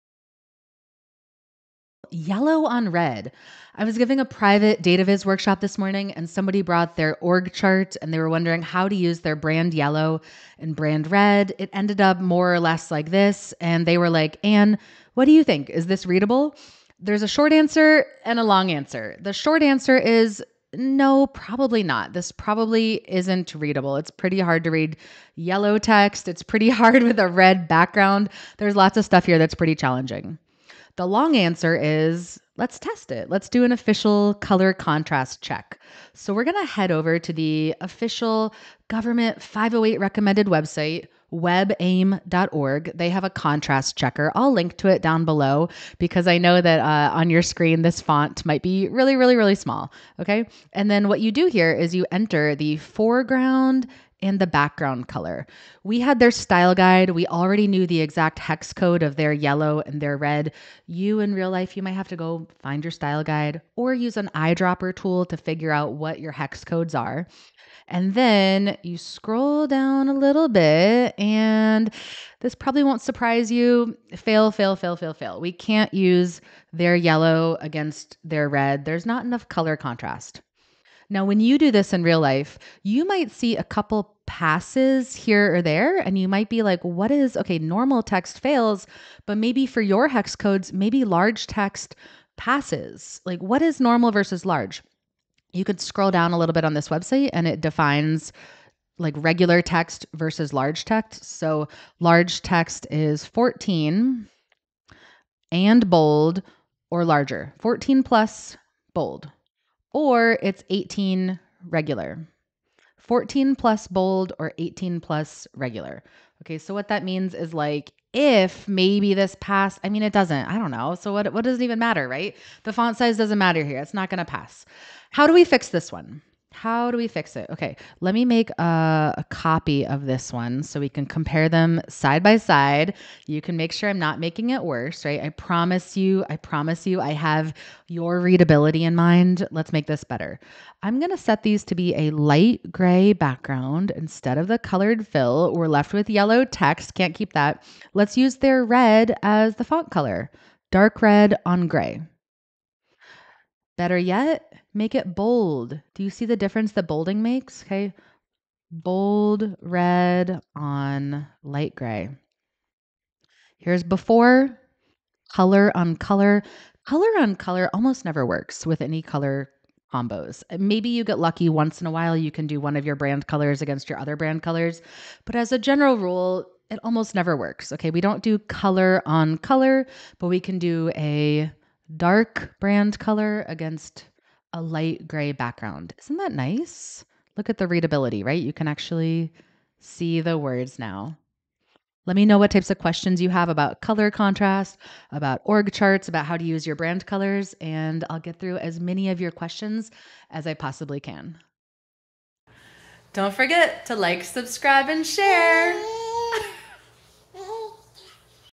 508 Compliance Rules for Graphs: Color Contrast Testing on Official Sites (Avoid Graphing Fines!)
Is "color on color" hard to read?

Short answer: YES.

You'll also see how to fix it.

0:00 Intro to Yellow on Red Example from Workshop
0:16 The Short Answer (Avoid Color on Color)
0:29 The Long Answer (Color Contrast Check)
1:35 Rules for Regular vs. Large Text
2:05 How Do We Fix It??
2:43 The Before-After Difference